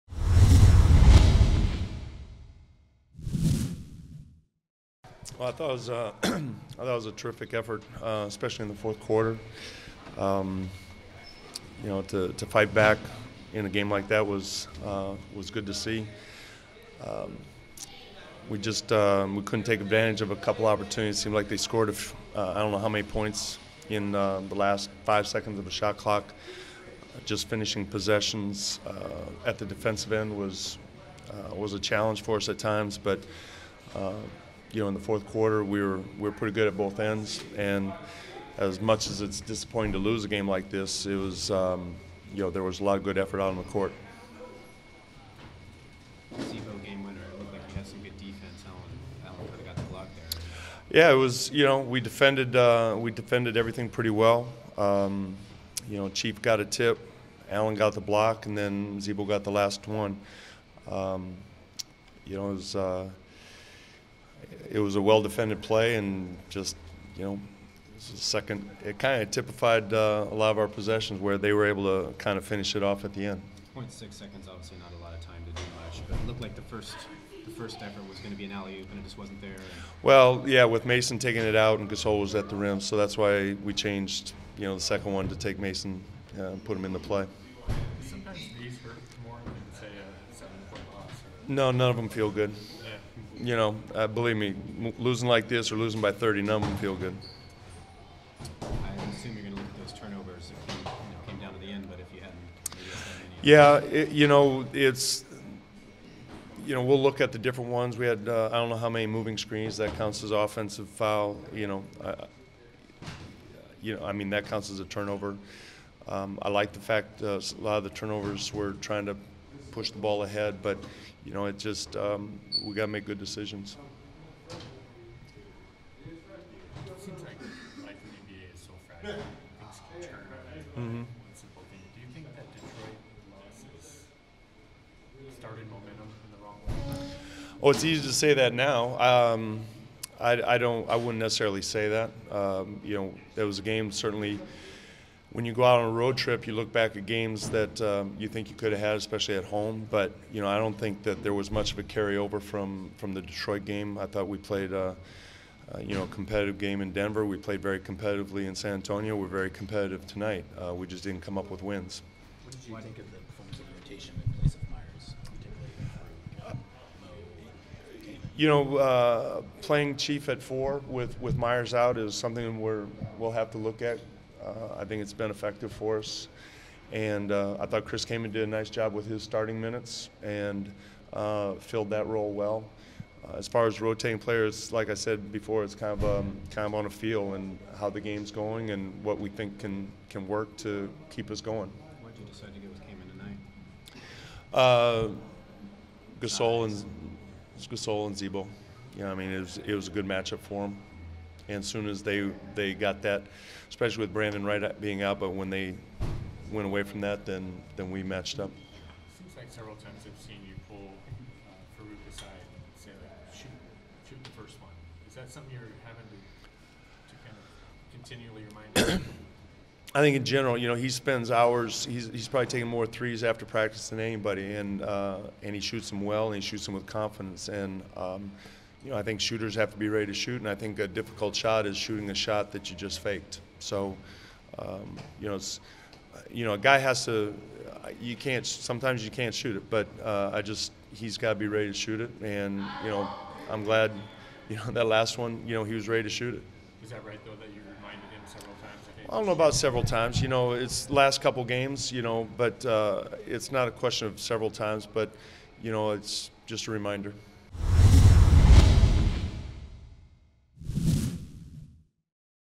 Terry Stotts Pleased with Effort Despite Loss in Memphis
Head coach Terry Stotts talks with reporters following Friday's defeat in Memphis.